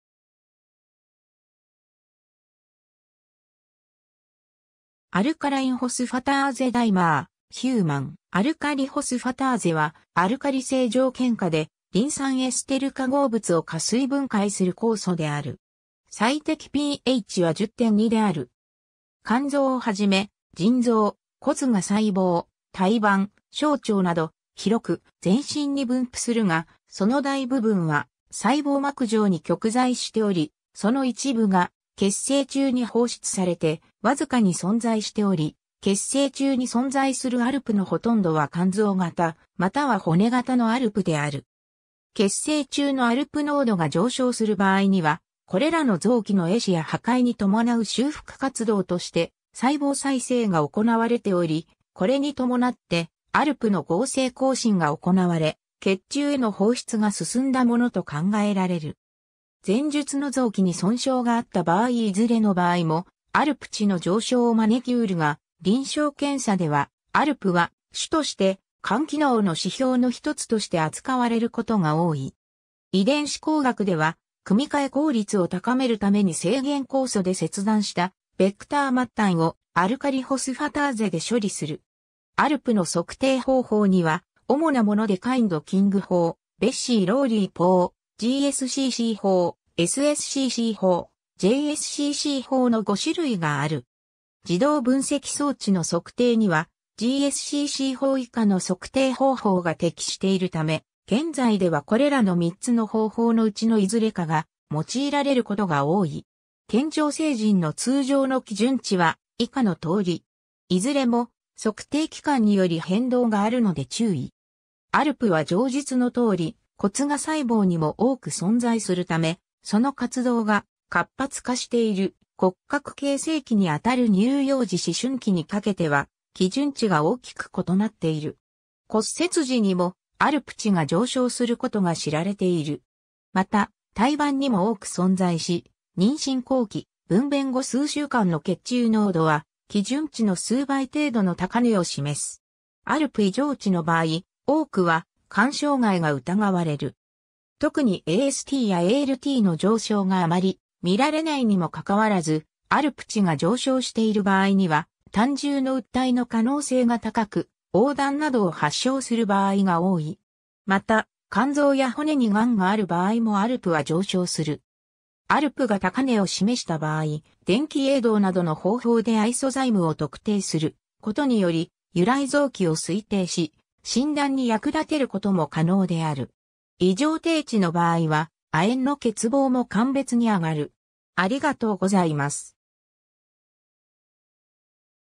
アルカリホスファターゼ
Alkaline phosphatase dimer, Human. アルカリホスファターゼ（Alkaline Phosphatase、略号:ALP；EC 3.1.3.1）はアルカリ性条件下でリン酸エステル化合物を加水分解する酵素である。
最適pHは10.2である。
 肝臓をはじめ、腎臓、骨芽細胞、胎盤、小腸など、広く全身に分布するが、その大部分は細胞膜上に局在しており、その一部が血清中に放出されて、わずかに存在しており、血清中に存在するALPのほとんどは肝臓型または骨型のALPである。
 血清中のALP濃度が上昇する場合には、これらの臓器の壊死や破壊に伴う修復活動として細胞再生が行われており、これに伴ってALPの合成亢進が行われ、血中への放出が進んだものと考えられる。
前述の臓器に損傷があった場合いずれの場合もALP値の上昇を招きうるが、臨床検査ではALPは主として肝機能の指標の一つとして扱われることが多い。
 遺伝子工学では、組み換え効率を高めるために制限酵素で切断したベクター末端をアルカリホスファターゼで処理する。
 ALPの測定方法には、主なものでKind-King法、Bessey-Lowry法、GSCC法、SSCC法、JSCC法の5種類がある。
自動分析装置の測定にはGSCC法以下の測定方法が適しているため、現在ではこれらの3つの方法の内のいずれかが用いられることが多い。
 健常成人の通常の基準値は以下の通り。
 いずれも測定機関により変動があるので注意。
 ALPは上述の通り、骨芽細胞にも多く存在するため、その活動が活発化している骨格形成期にあたる乳幼児～思春期にかけては基準値が大きく異なっている。
骨折時にもALP値が上昇することが知られている。
また、胎盤にも多く存在し、妊娠後期～分娩後数週間の血中濃度は基準値の数倍程度の高値を示す。
 ALP異常値の場合、多くは肝障害が疑われる。
特にASTやALTの上昇があまり見られないにもかかわらずALP値が上昇している場合には胆汁の鬱滞の可能性が高く、黄疸などを発症する場合が多い。
また、肝臓や骨に癌がある場合もALPは上昇する。
ALPが高値を示した場合、電気泳動などの方法でアイソザイムを特定することにより由来臓器を推定し、診断に役立てることも可能である。
 異常低値の場合は亜鉛の欠乏も鑑別に上がる。